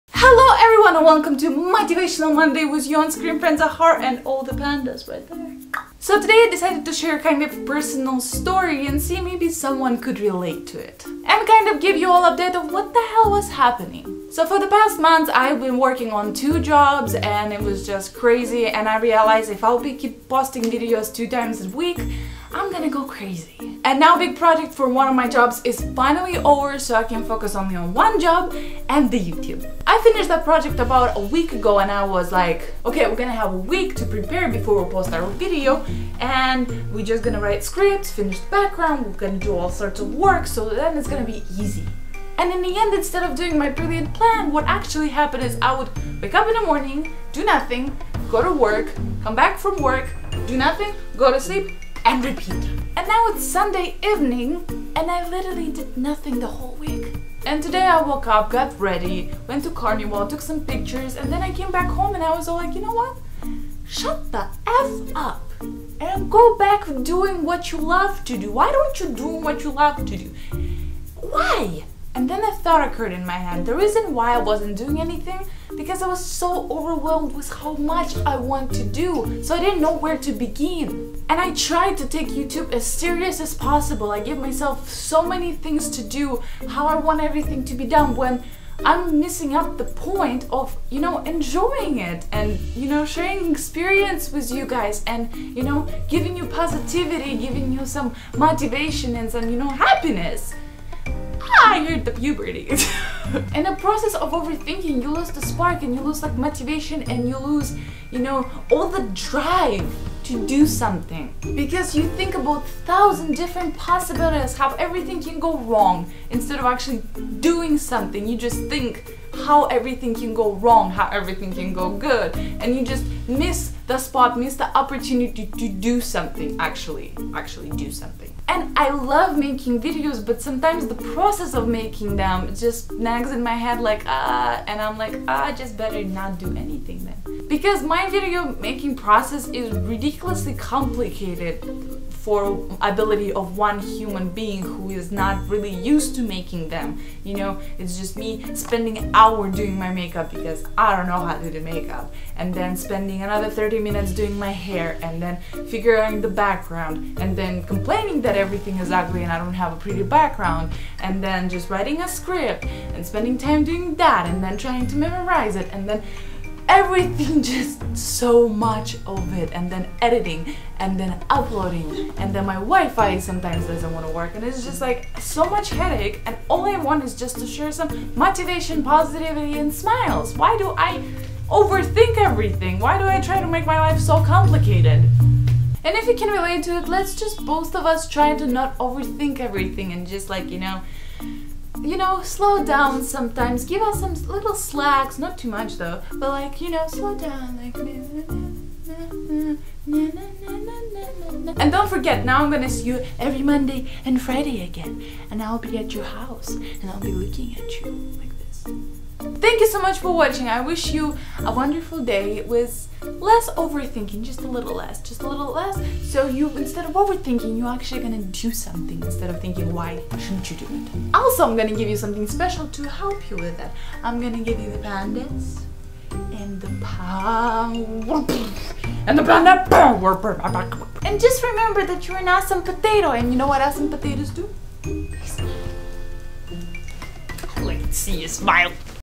Overthinking Rant
To all of people who overthink - don't do it to the extreme.

PANDA POWER with you! And just remember, you are an AWESOME POTATOE 😀

And have a wonderful day today filled with great events, people and hard work that makes you happy!